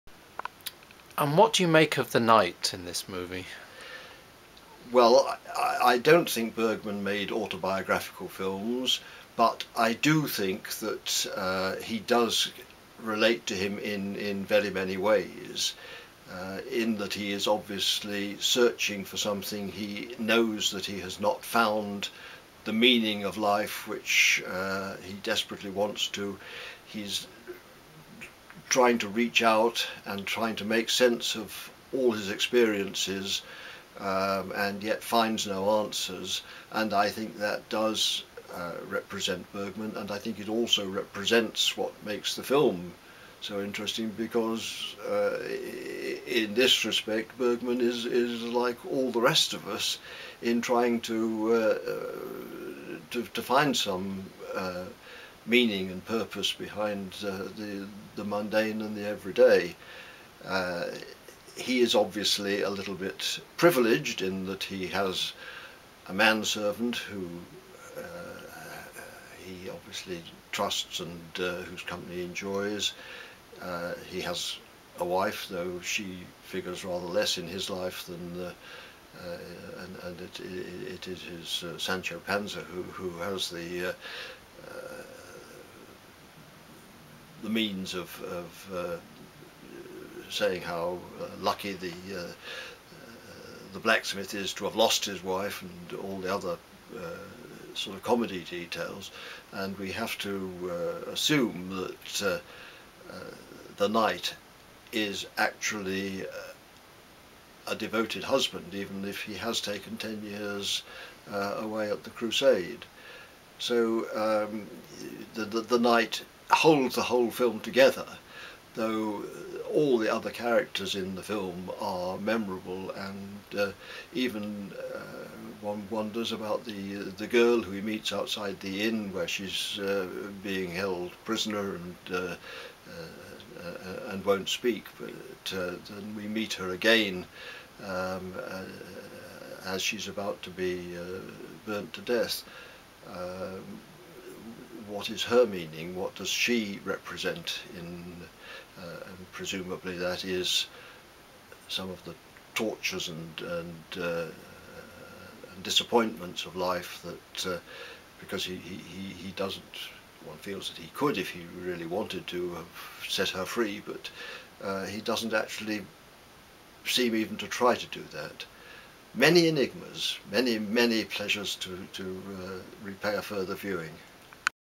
Timothy Gee Bergman Seventh Seal the Knight 03
Timothy Gee talking about the role of theKnight in Bergman's Seventh Seal.

Talking to Michael Noonan 5th February 2013 in London.